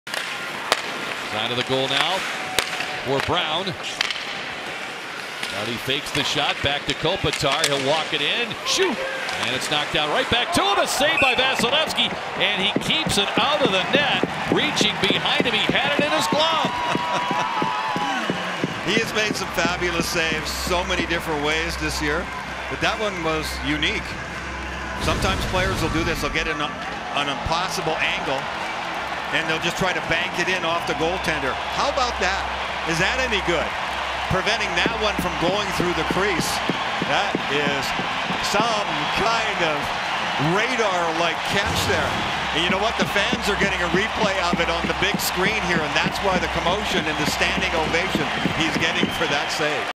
Vasilevskiy make Crazy (Behind-The-Back) save [Kopitar Shouts]
Andrei Vasilevskiy displays staggering athleticism in net as he extends his glove behind his back for a stunning glove save on Anze Kopitar! You Don't Want To Miss This Save!
This save Might be a candidate for Save-of-the-year in NHL